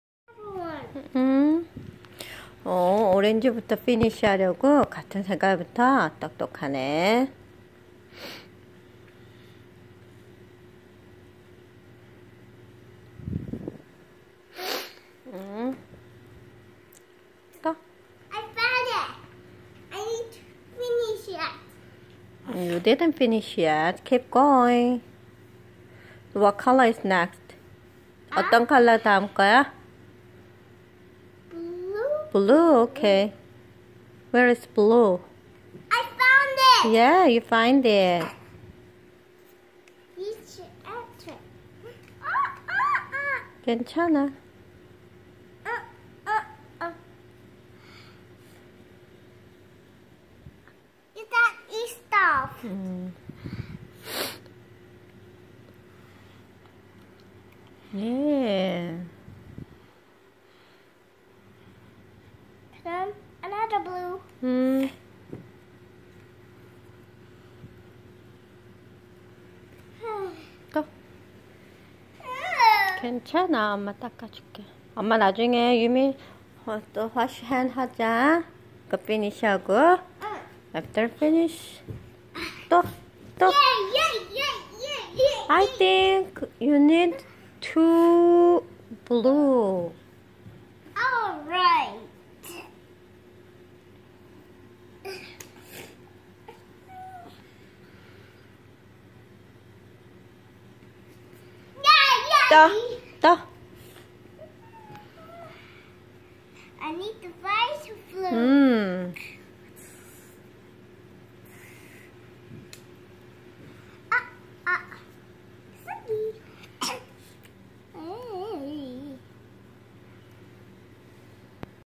# 3살 7개월 유미양 # 1312days # 색깔 맞춰 색 종이 붙이기
그냥 밑에 칼라만 정해놓구. 붙이라고 햇더니, 어머나!!! 그냥 보이는데로 붙이지않고, 칼라 선정을 한후에 그거 다끝나면 또다릉 칼라를 붙이던 유미양.